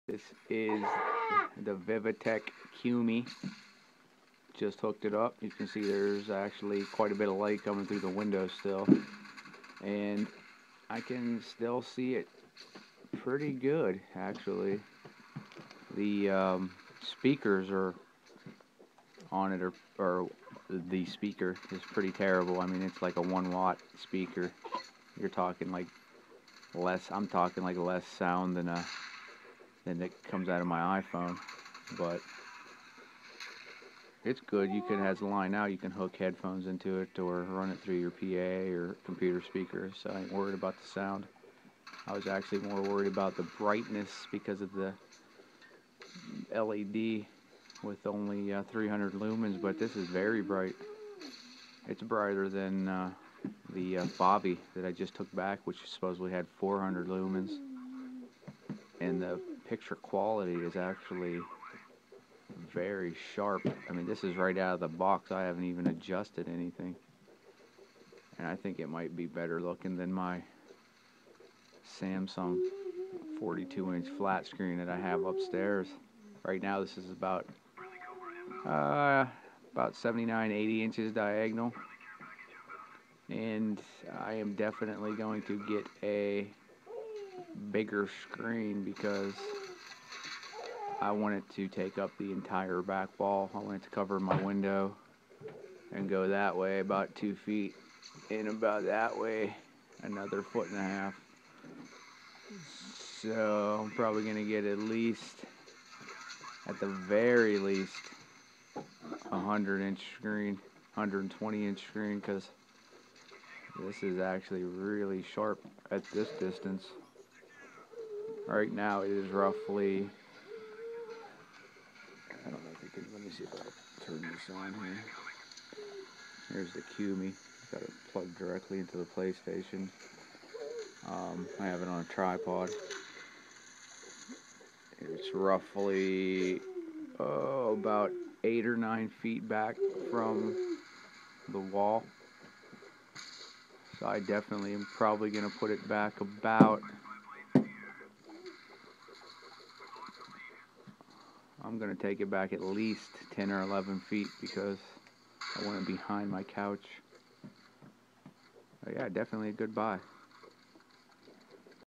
vivitek qumi q2
unboxing and hooked up to my ps3, looks great about 9 feet from my wall